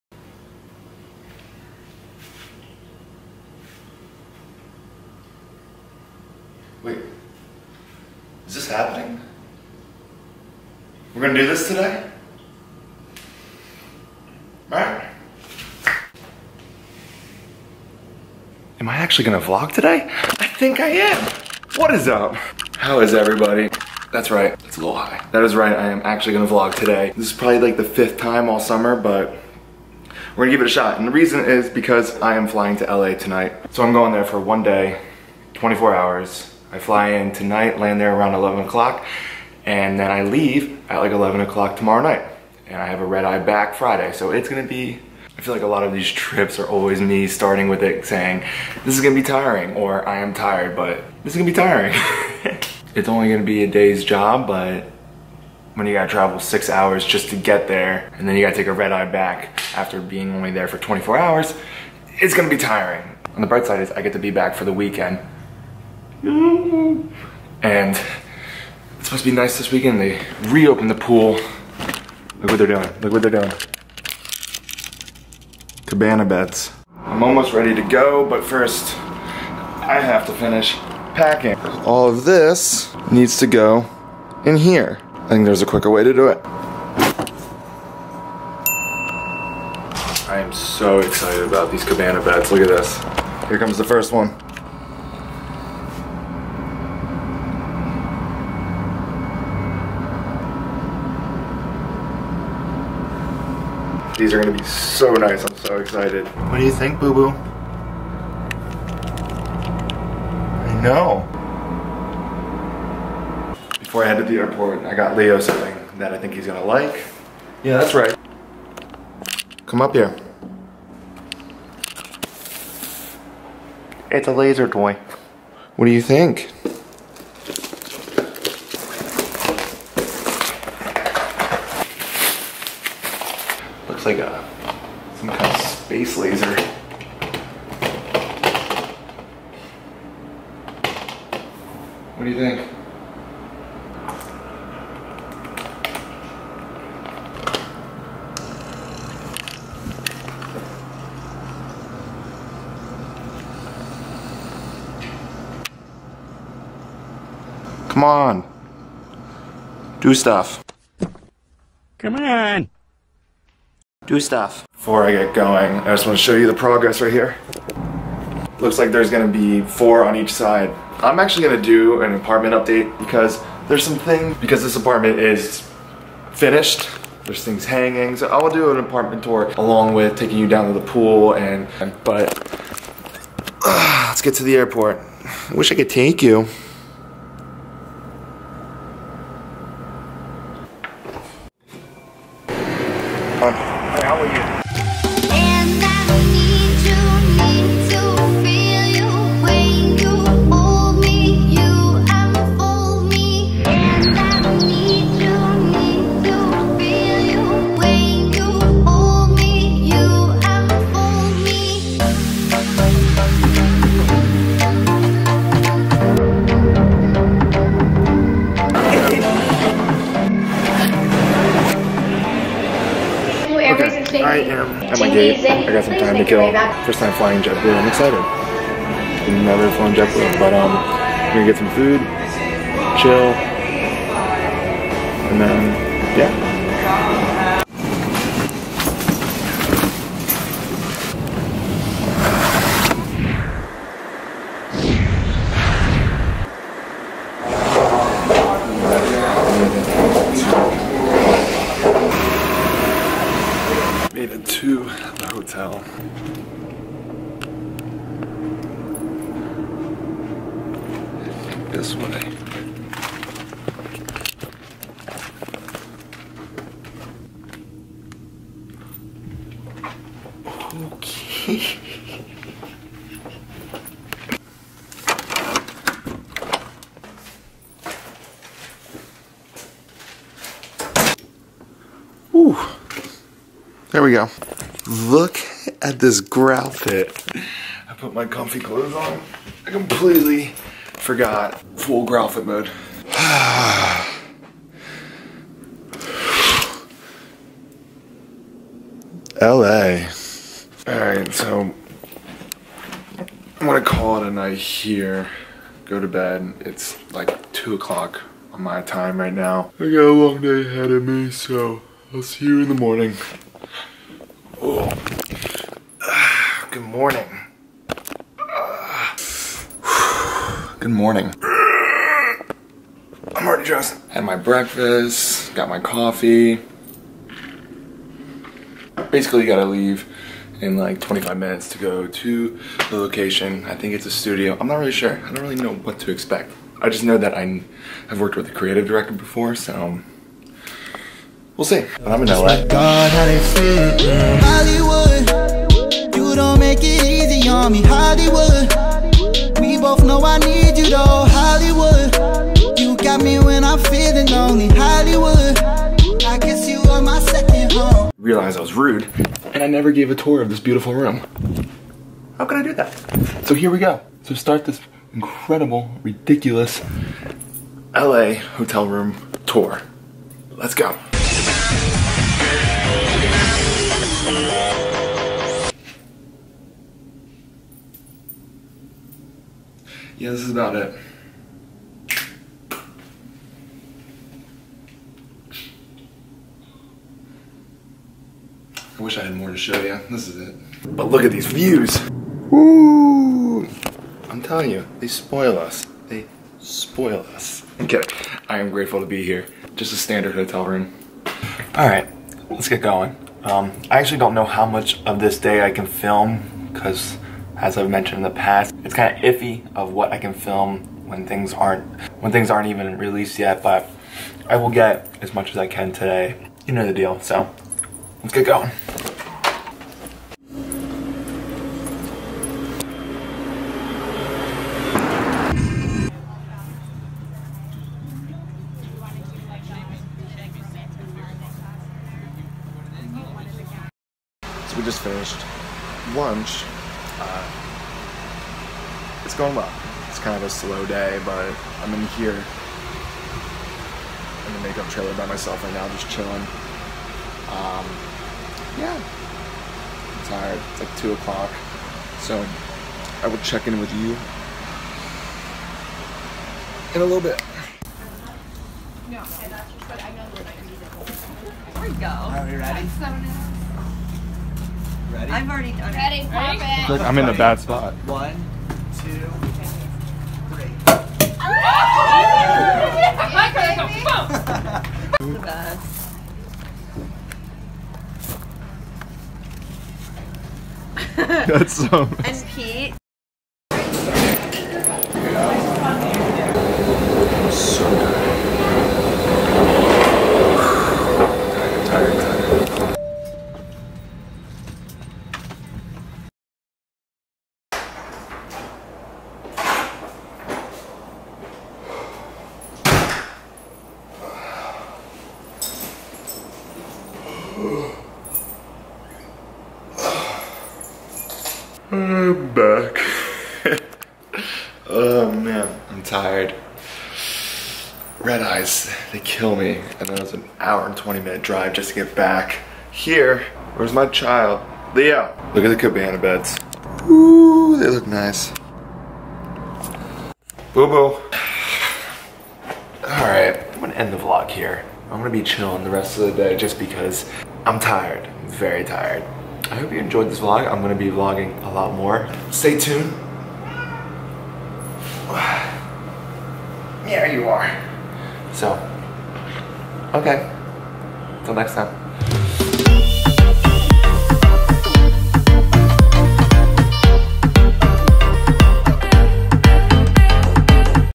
24 Hours In LA
Had to fly to LA for 24 hours for a photoshoot. Although tiring it was an awesome time and fun shoot!